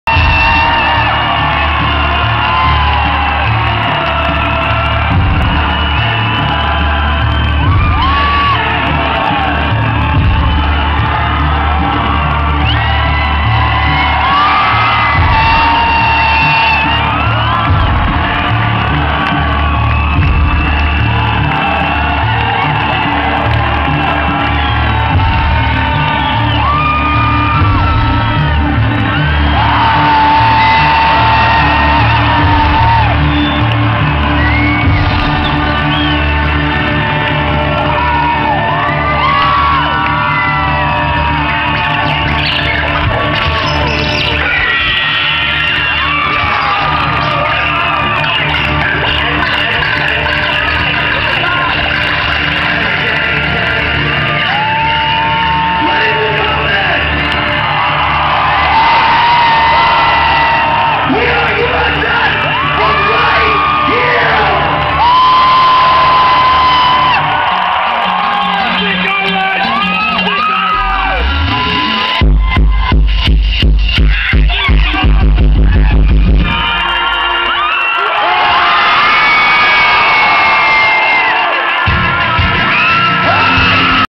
USS hitting the stage at the CNE
Ubiquitous Synergy Seeker at the CNE Bandshell. I'm so pissed at myself that I stopped recording when I did! Right after this video ends, the torches were lit off haha. I didn't know so I missed it :( Anyways, here they are starting their show at the CNE. Taken on August 24th 2013. Toronto, Ontario.